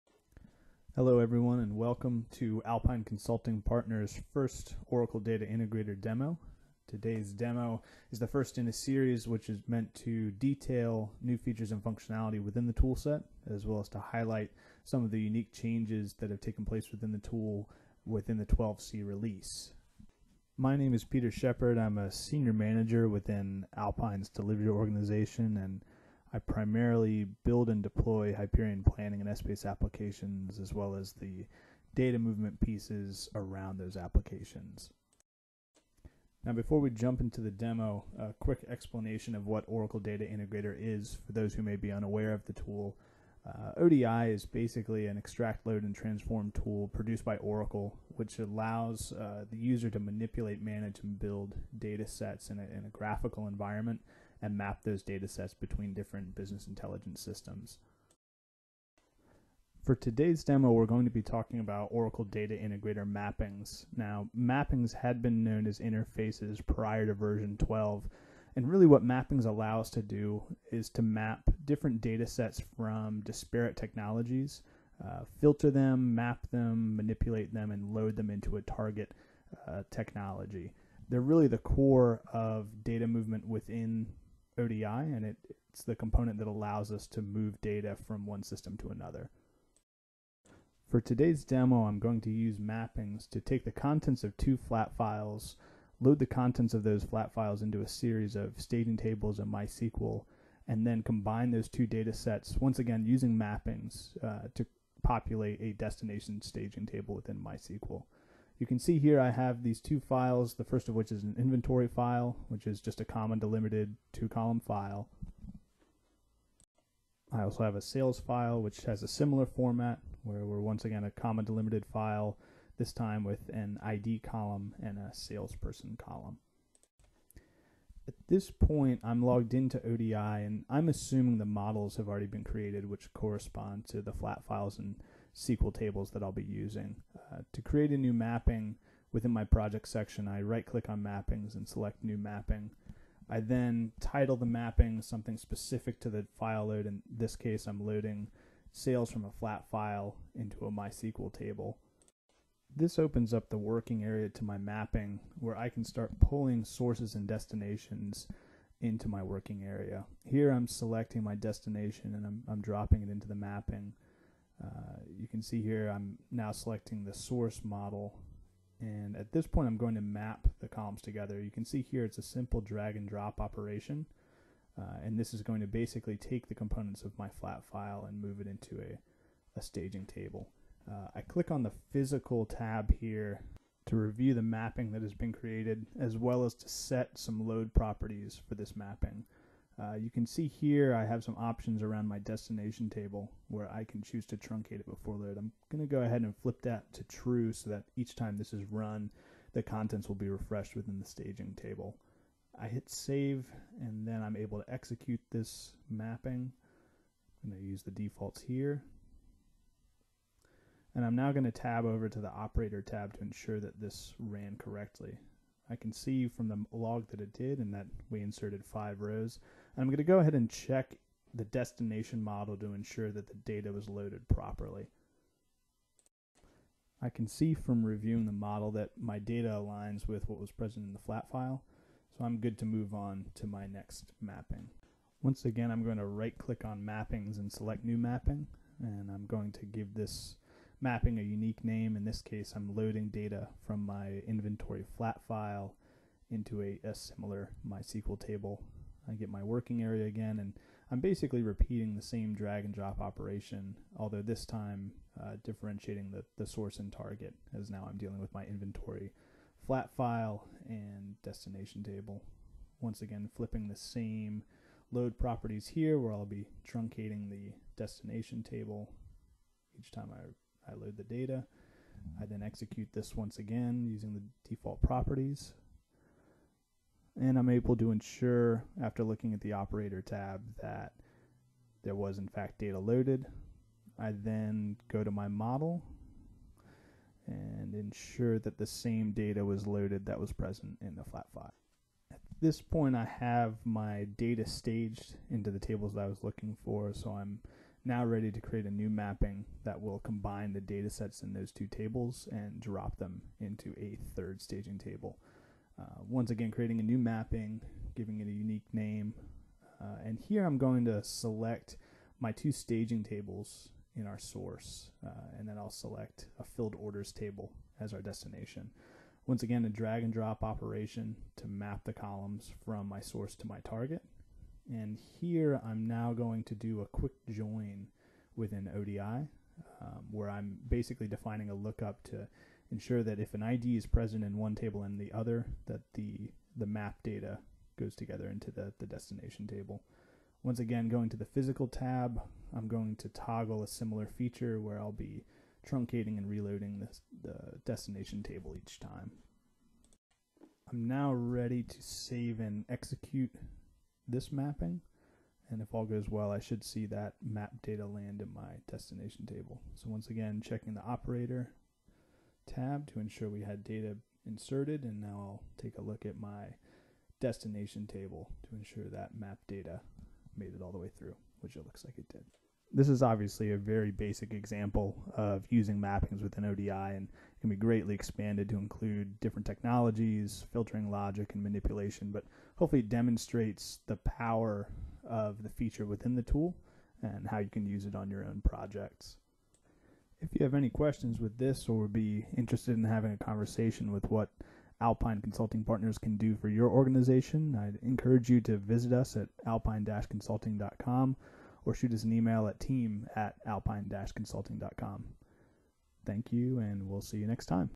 ODI Demo Series - Session 1 - ODI Mappings
In this session we take you through the usage of the Mapping technology and guide you through the configuration in a simple example.